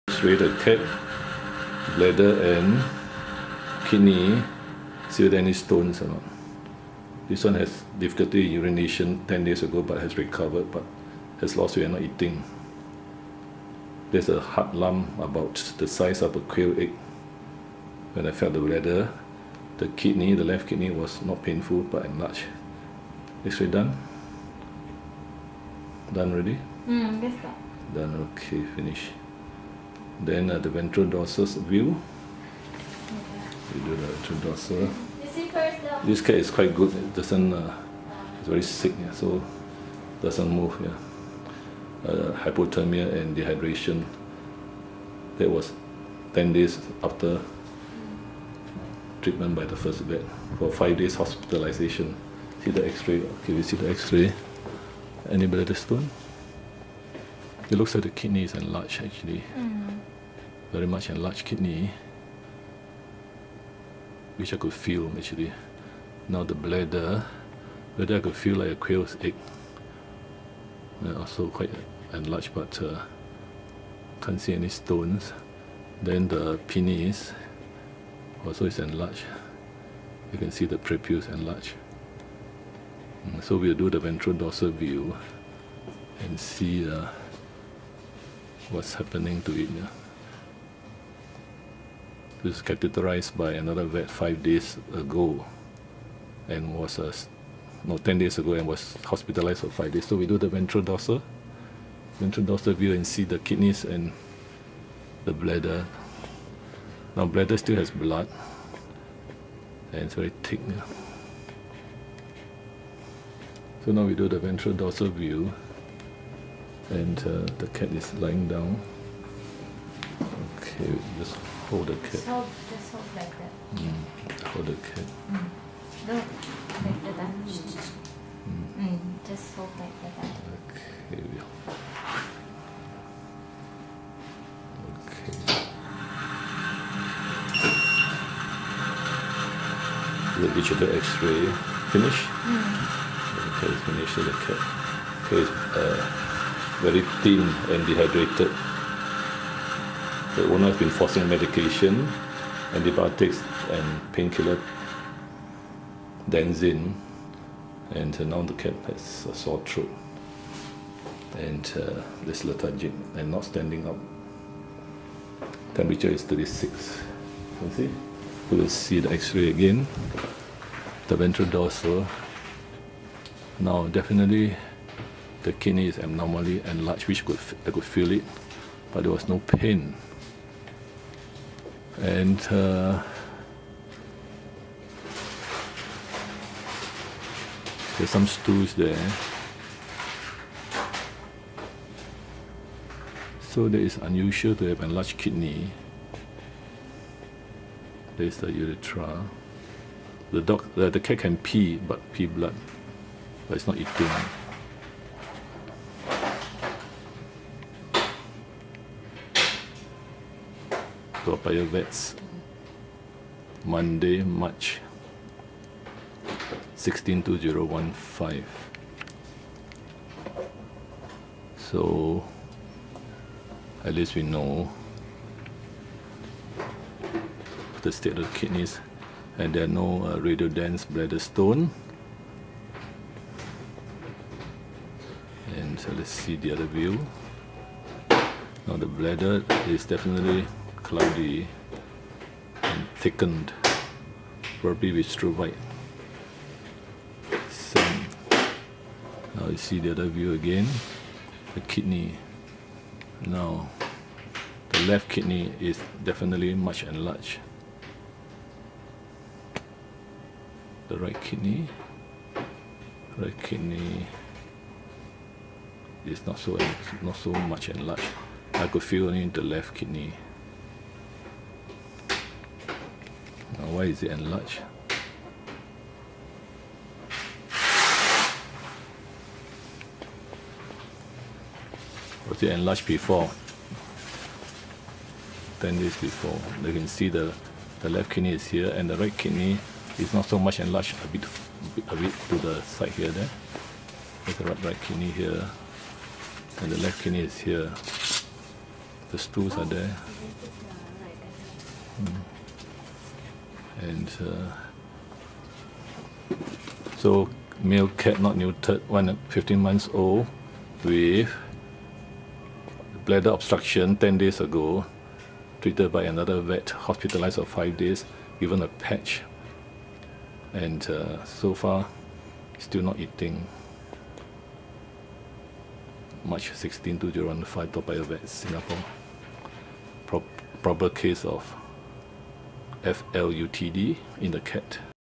The cat is X-rayed Pt 2
The X-rays show abnormally enlarged kidney. The cat is being X-rayed. The bald patch on the skin was due to the other vet putitng on a patch.